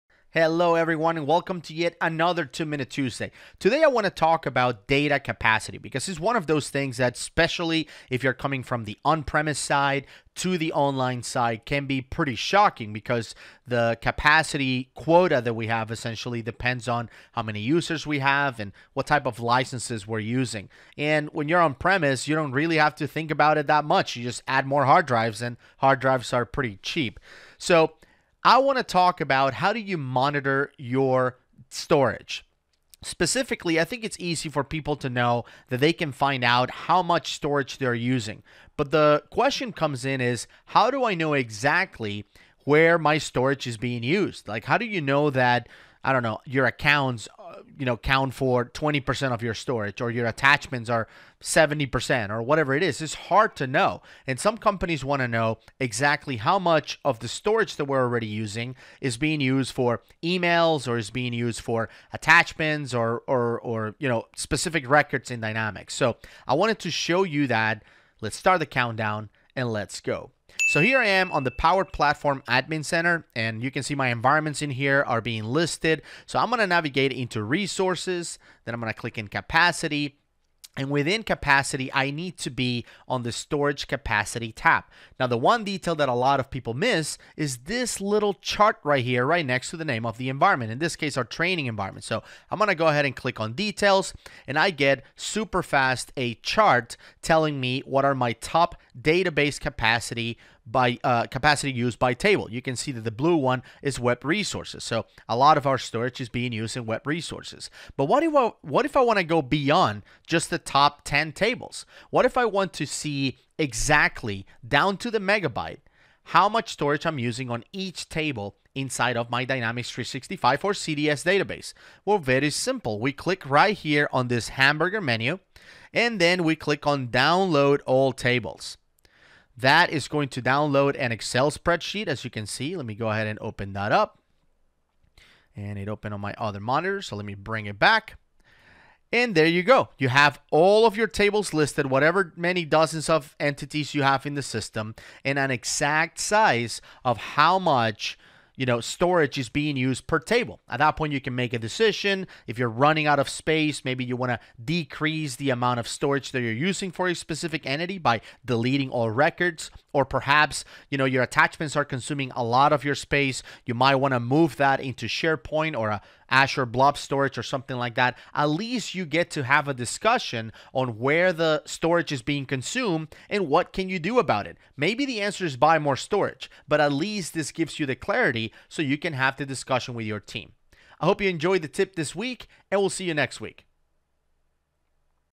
Dynamics 365 2MT Episode 150: GETTING A DETAILED DATABASE CAPACITY USE BY TABLE REPORT
It is very important to have the ability to understand how your storage is being used.

Here's the best way to make that happen.